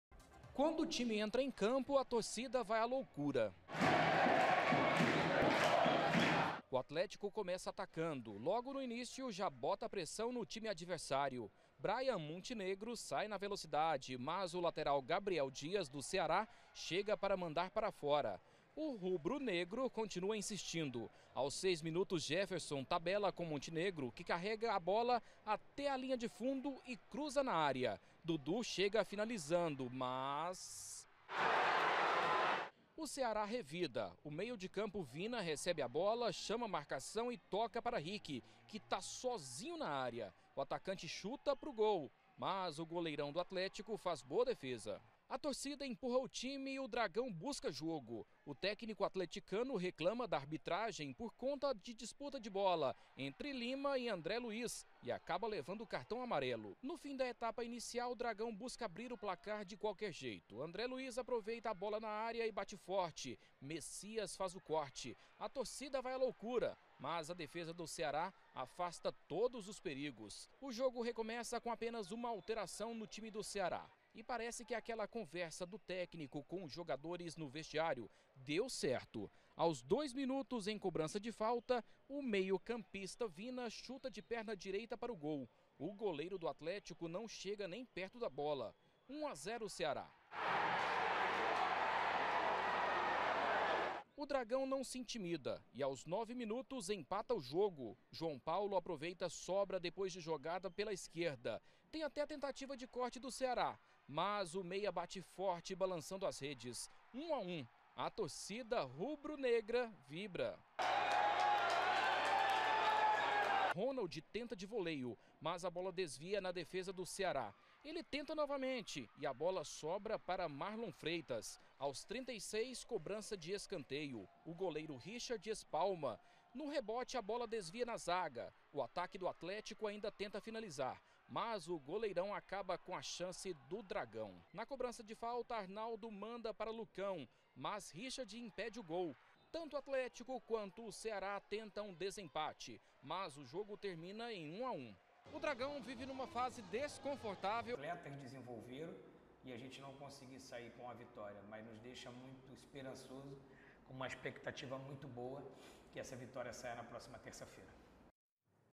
NO SUFOCO! DRAGÃO SEGUE SEM VENCER E ESTÁ HÁ 3 PONTOS DO Z4
ATLÉTICO. DRAGÃO SEGUE SEM VENCER. ATÉ QUE FEZ UM BOM JOGO DIANTE DO CEARÁ, SAIU ATRÁZ BUSCOU O EMPATE, MAS A VITÓRIA NÃO VEIO E A SITUAÇÃO CONTINUA PERIGOSA. O TIME ESTÁ SÓ HÁ TRÊS PONTOS DA ZONA DE REBAIXAMENTO.